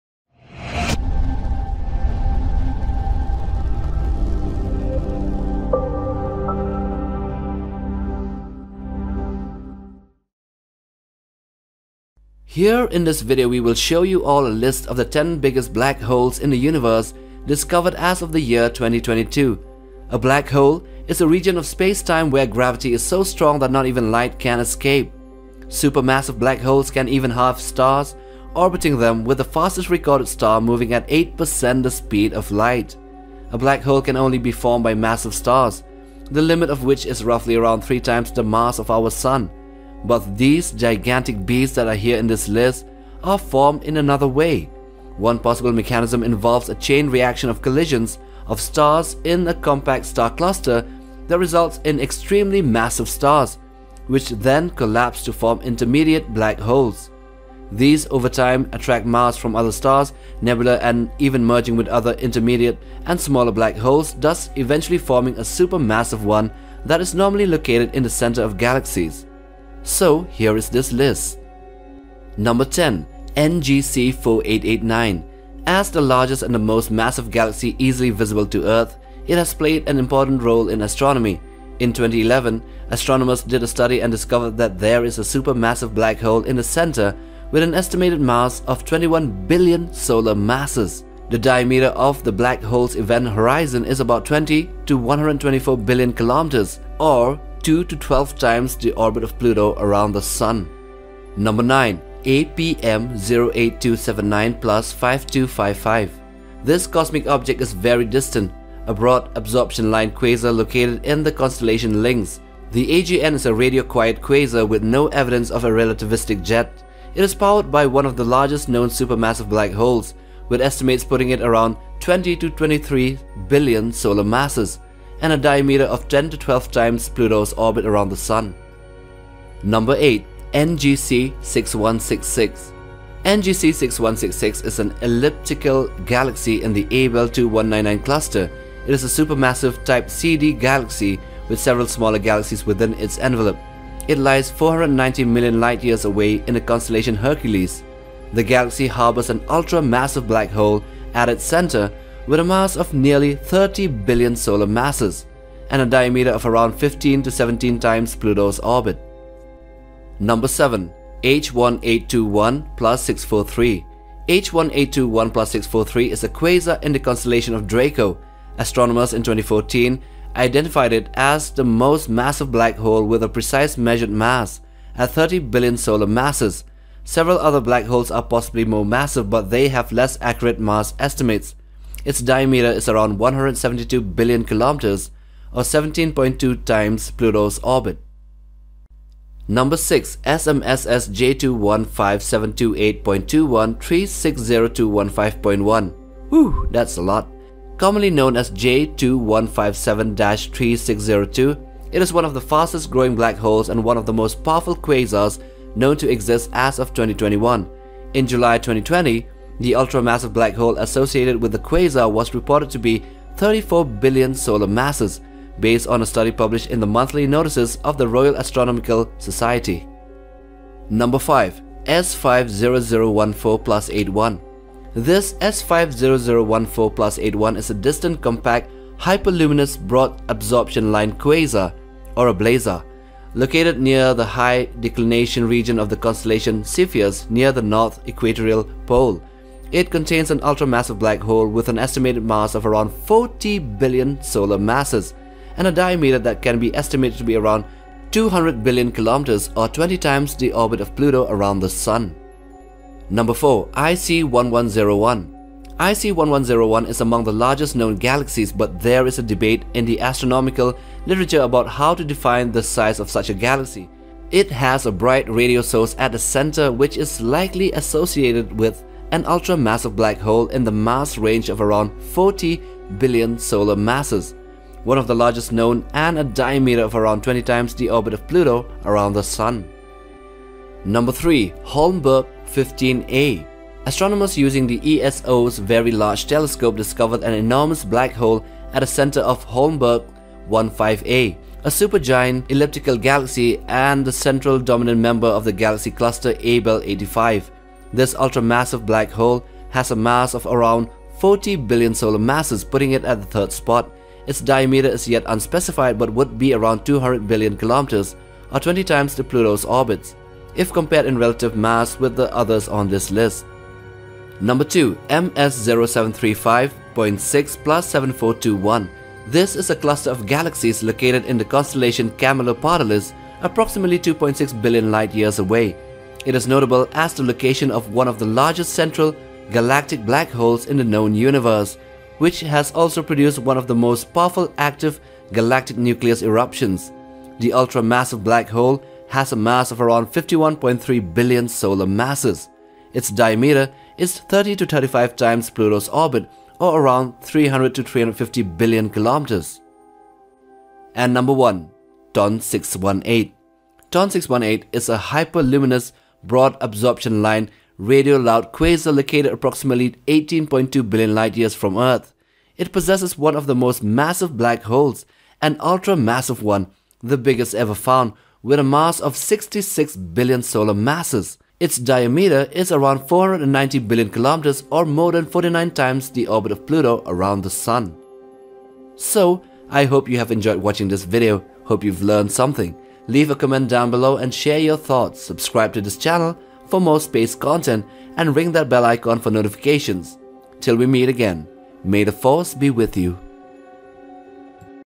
10 Biggest Black Holes Ever Discovered (2022)
A black hole is a place in space where gravity pulls so much that even light can not get out. Gravity is so strong because matter has been squeezed into a tiny space. This can happen when a star is dying. Because no light can get out, people can't see black holes. They are invisible. Space telescopes with special tools can help find black holes. The special tools can see how stars that are very close to black holes act differently than other stars. We have compiled a list of the 10 Biggest Black Holes ever discovered based on reliable resources. Enjoy the video guys!